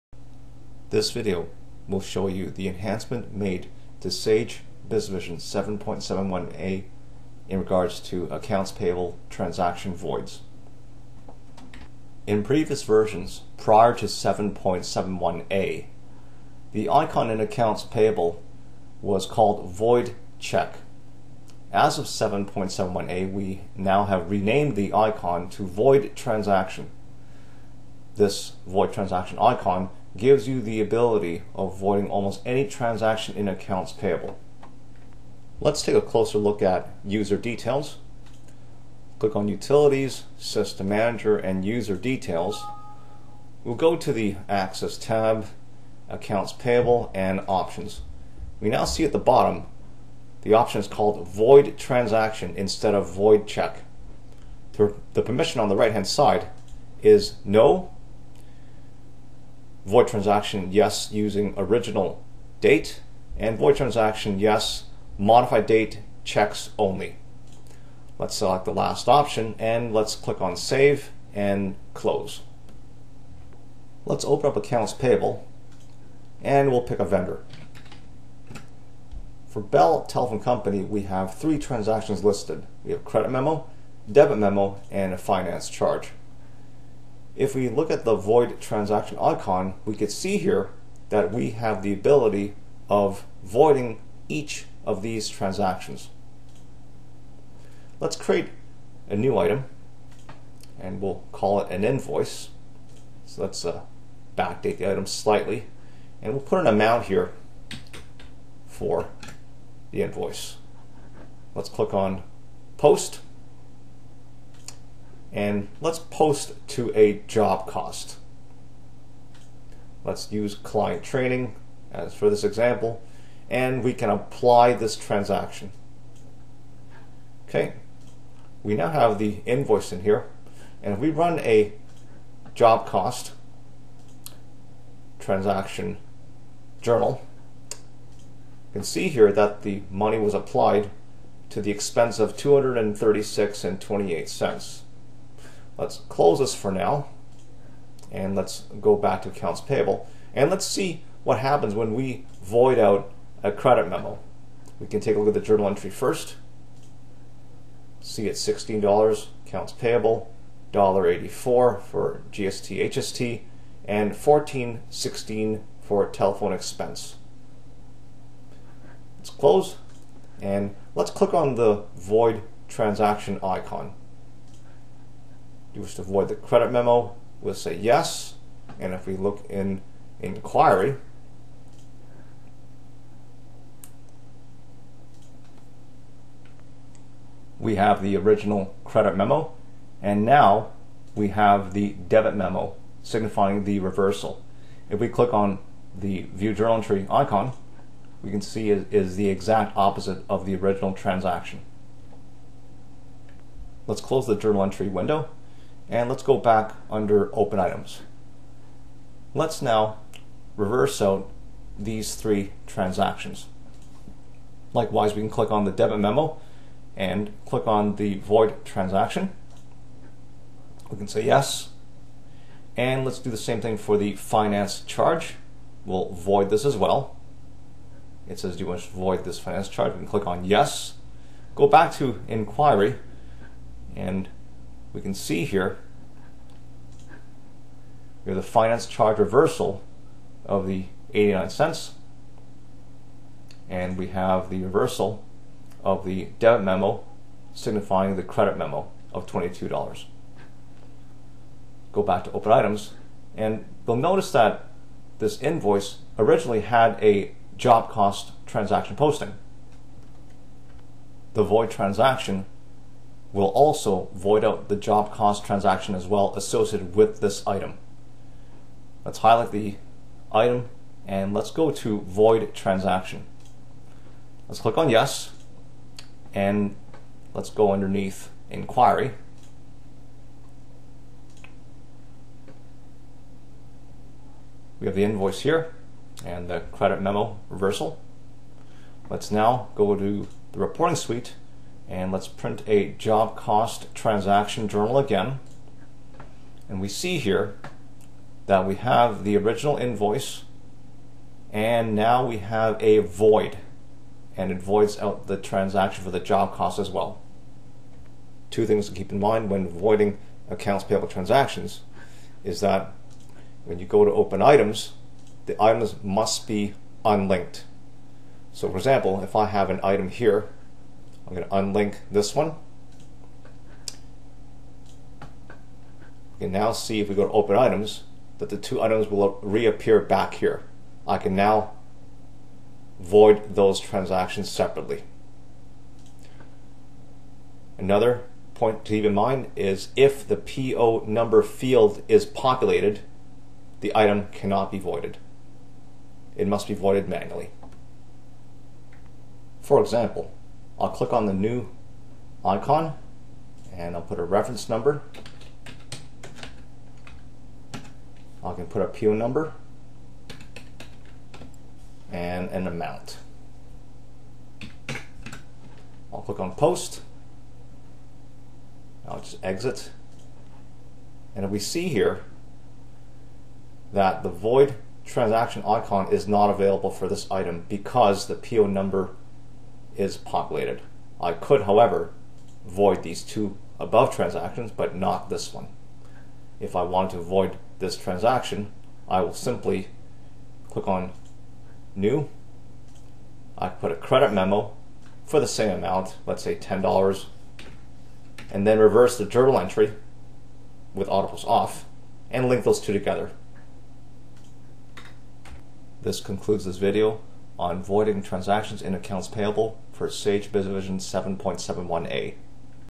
Sage BusinessVision 2014 Update 1 New Features: Void Transaction
Show you how to use the void transaction feature in Account Payable.